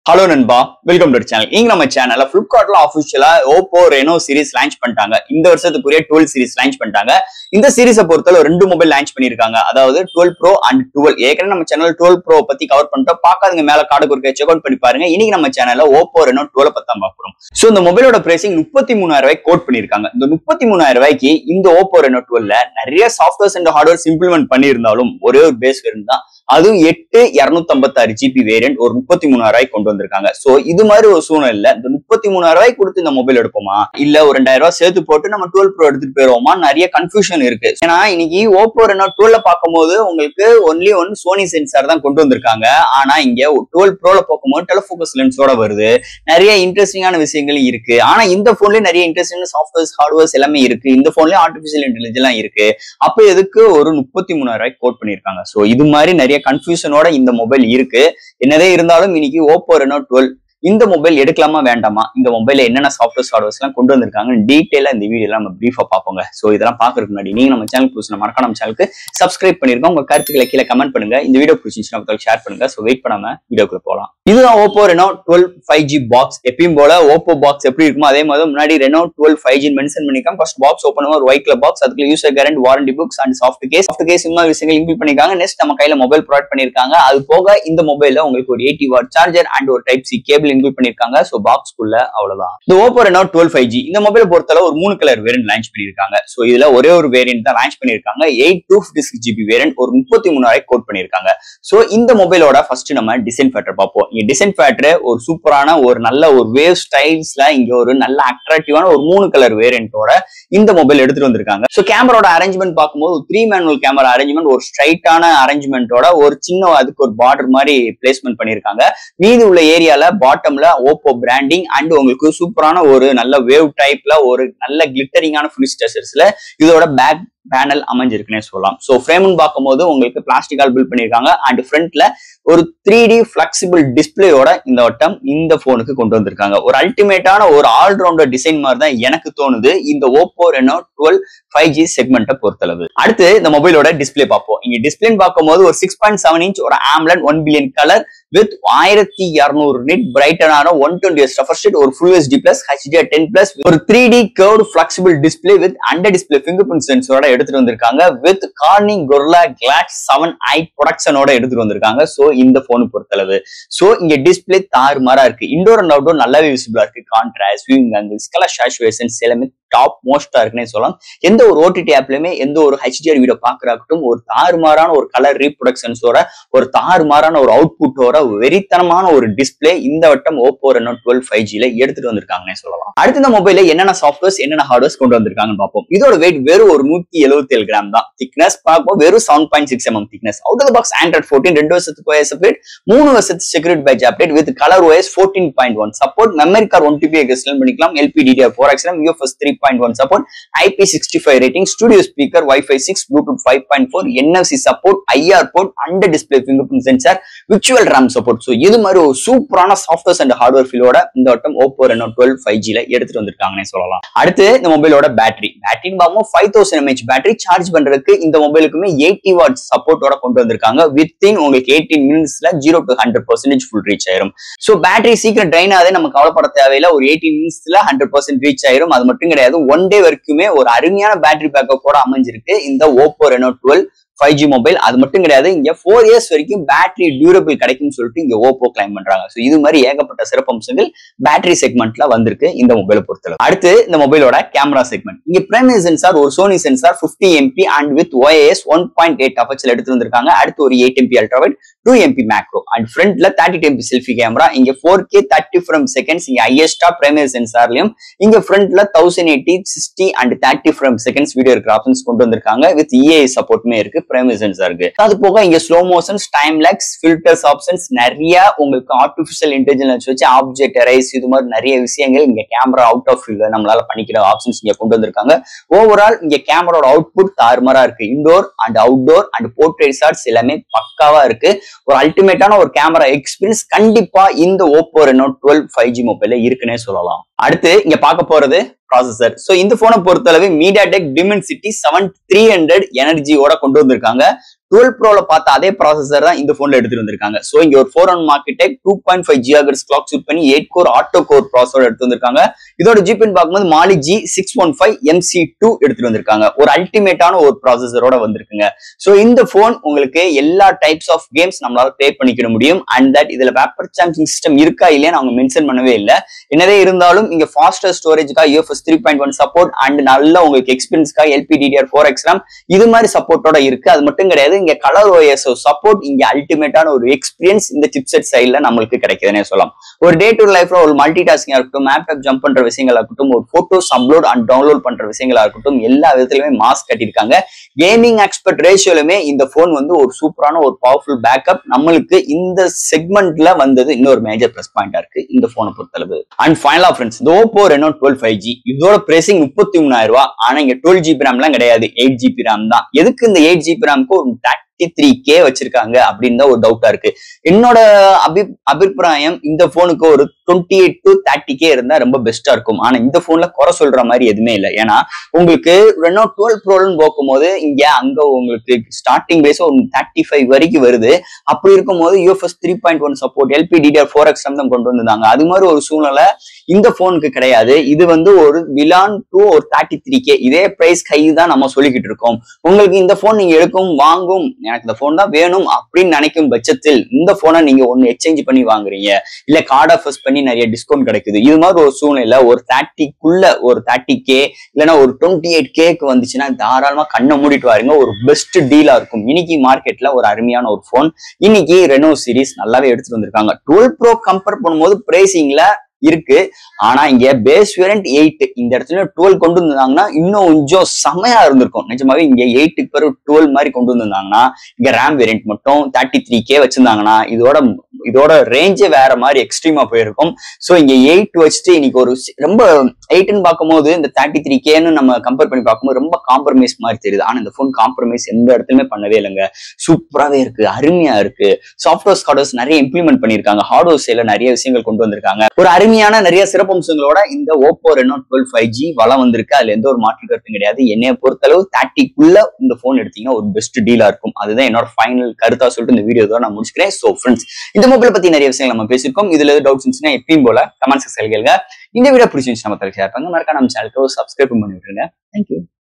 OPPO RENO 12 5G - Unboxing & Review - Tamil
OPPO RENO 12 5G .First Impression in Tamil Tech. Watch the video fully and share your thoughts about this smartphone with me in comments.